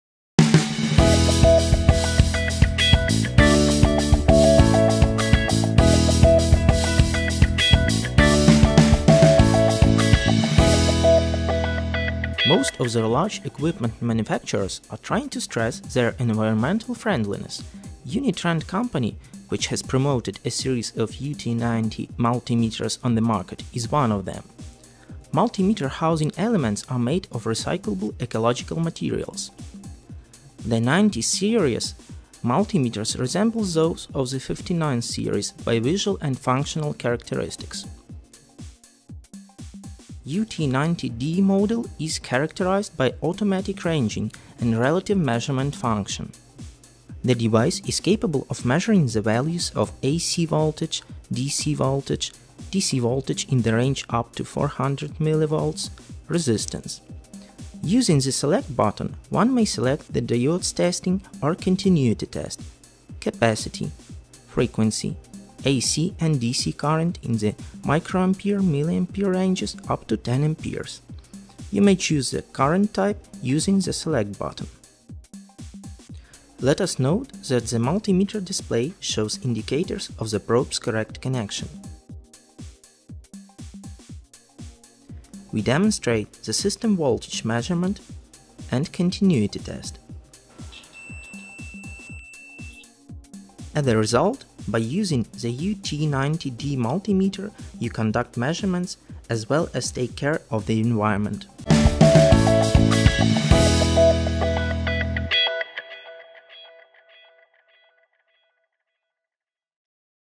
UNI-T UT90D Digital Multimeter
Many large manufacturers hardly care about the environment. UNI-TREND, the manufacturer behind the UT90 series multimeters, definitely isn't one of them. This is why the housing components of the brand's multimeters are made of recyclable ecological materials.
In both visual and functional terms, the multimeters of the UT90 series are similar to those of the UT59 series. The UT90D model also provides automatic ranging and relative measurement function.

You can use the device for measuring:
• AC voltage
• DC voltage
• DC voltage of up to 400 mV
• resistance
Press the Select button to select either of the below
• diodes testing or continuity test
• capacity
• frequency
• AC and DC current in the µA and mA ranges up to 10 A.
You may also choose the current type using the Select button.

The indicators of test leads connection are shown at multimeter display.
In our video, we carry out the measurement of voltage and continuity test.

Basically, in using the UT90D multimeter you can both take measurements you need and take care about the environment.